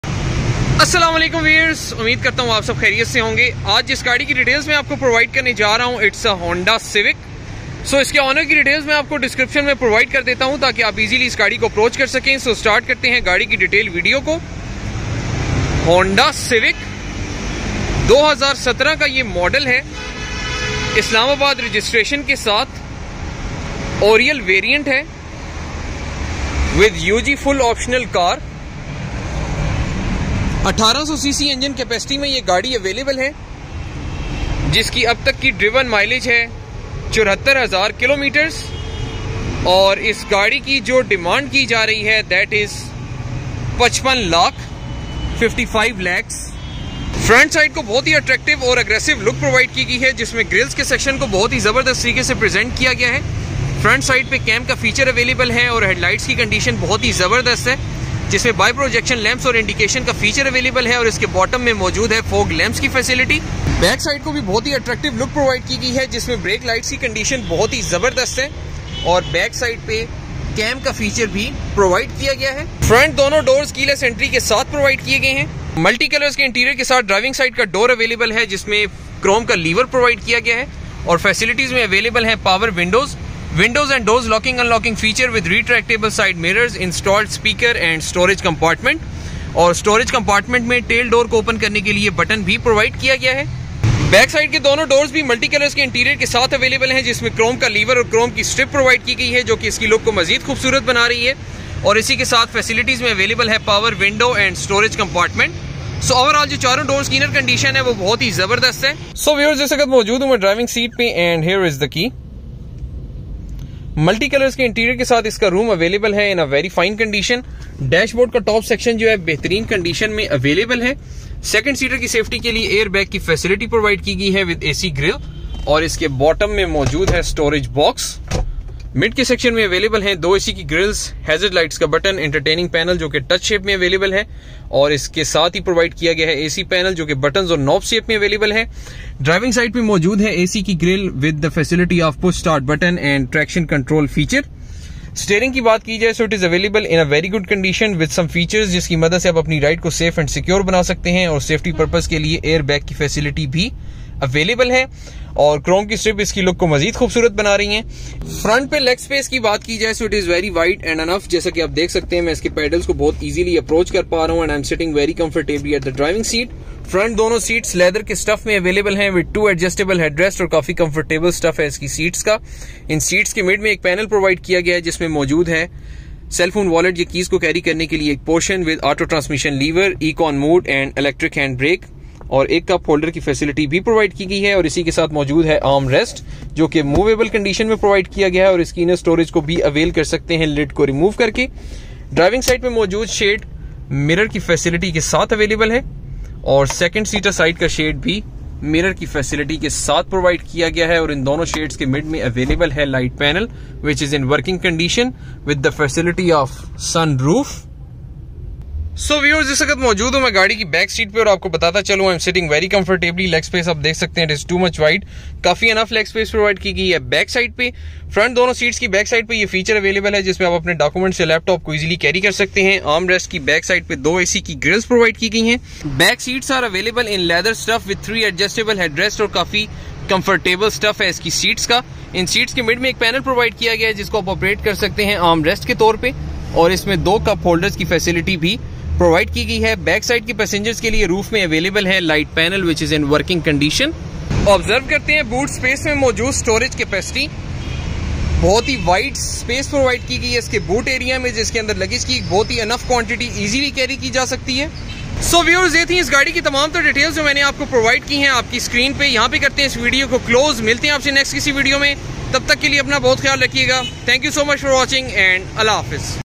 Honda Civic 1800 cc Oriel With Full Optional, Automatic With SunRoof, Isd Reg 2017 Model
New Deen Motors, Main Murree Road, Sadar, Rawalpindi
Contact With